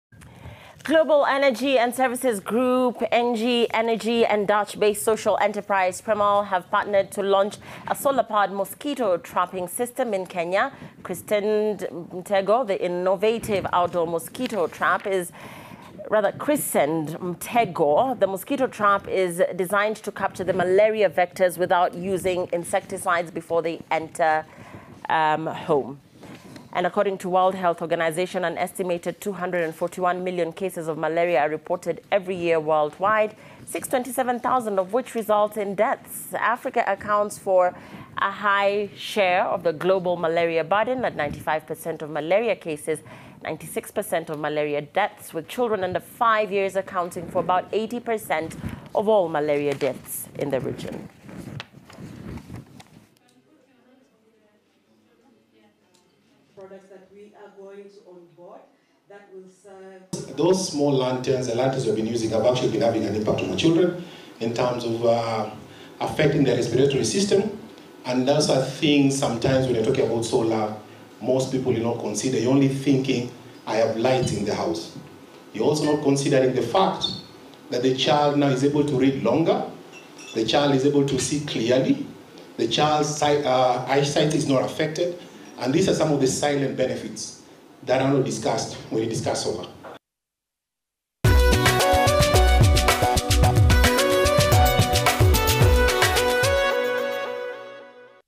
Solar powered mosquito traps launched
Malaria causes enormous drain on African economies